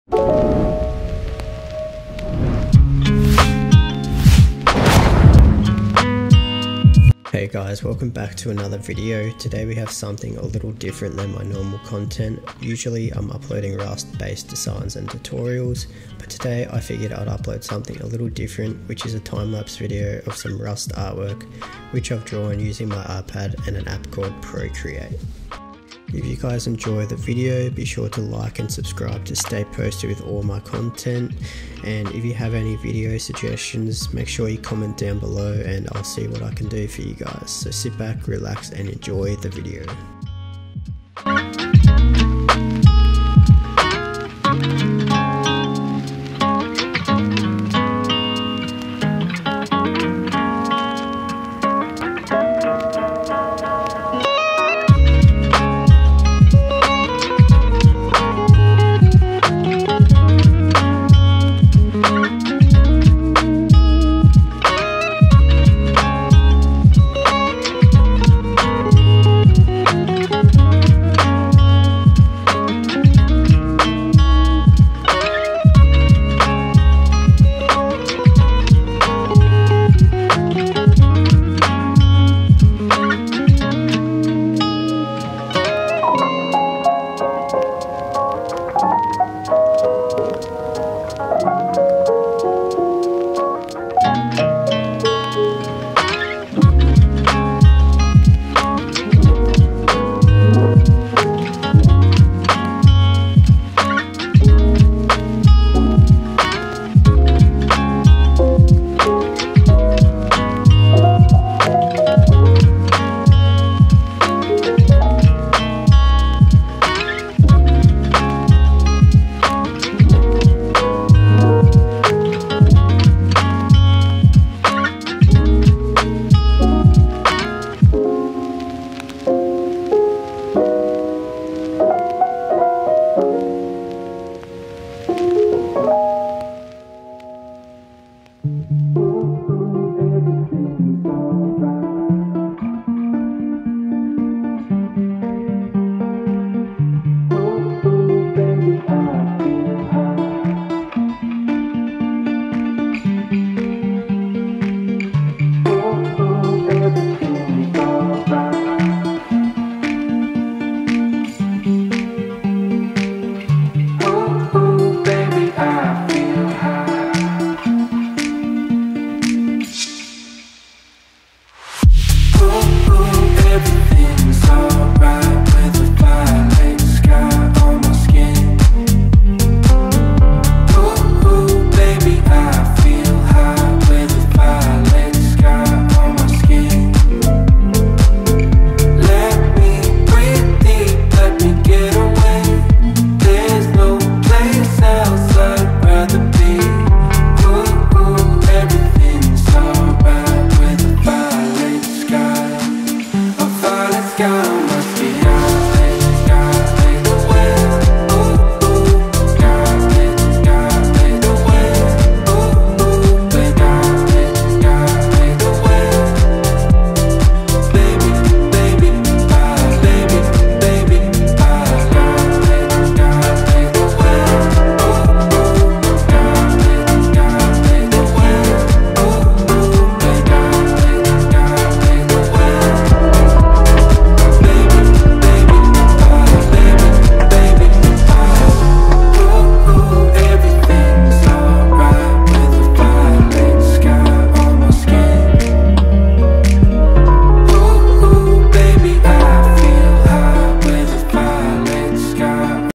Rust Thumbnail Speed Art Drawing Using iPad Procreate 2022 Time-lapse
So you want to relax and watch some Rust inspired artwork being created. Here I've hand drawn some rust artwork using my iPad and an App called Procreate. This art was hand sketched and drawn using an apple pen.

SOCIALS